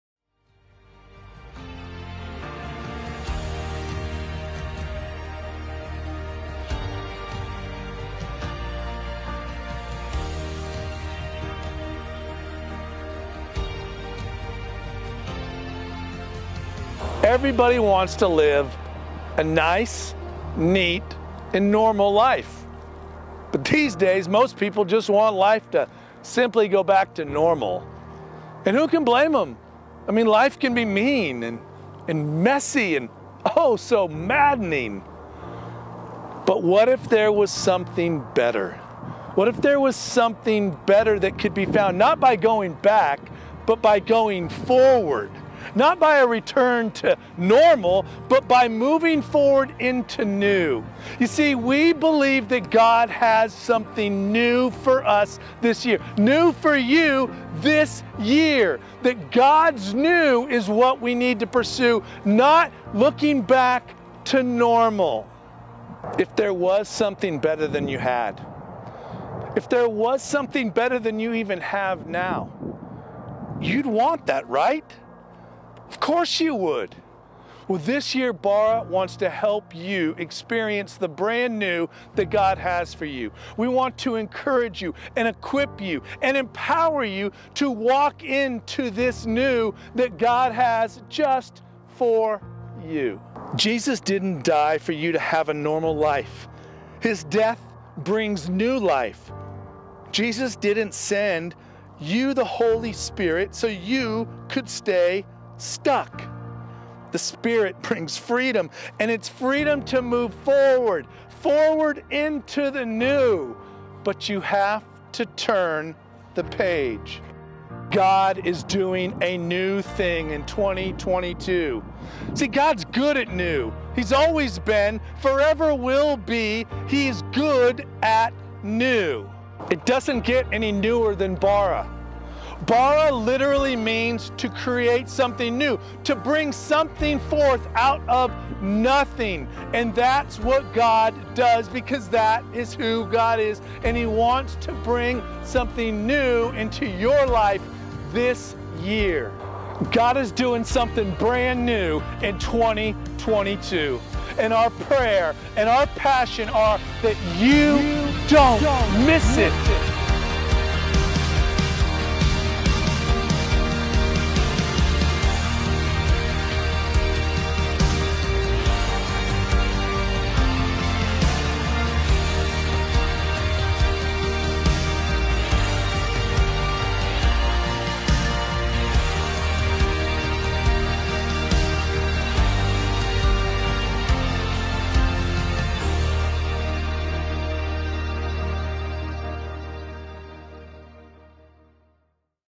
NEW Intro Video 2022
"Forget about what's happened; don't keep going over old history.
 Be alert, be present. I'm about to do something brand-new.
It's bursting out! Don't you see it? There it is!” – Isaiah 43:18-19 The Message

Everybody wants to live a nice, neat, and normal life.

But these days, most people just want life simply to go back to normal.

And who can blame them? I mean life can be mean, and messy, and oh so maddening.

But what if there was something better?

What if there was something better that could be found not by going back but by going forward?

Not by a return to normal, but by moving forward into NEW.

You see we believe that God has something NEW for us this year.

NEW for YOU this year.

That God’s NEW is what we need to pursue, not looking back to normal.

If there was something better than you had, if there was something better than you even have now, you’d want that, right?

Of course, you would.

This year Bara wants to help you experience the brand NEW that God has for you!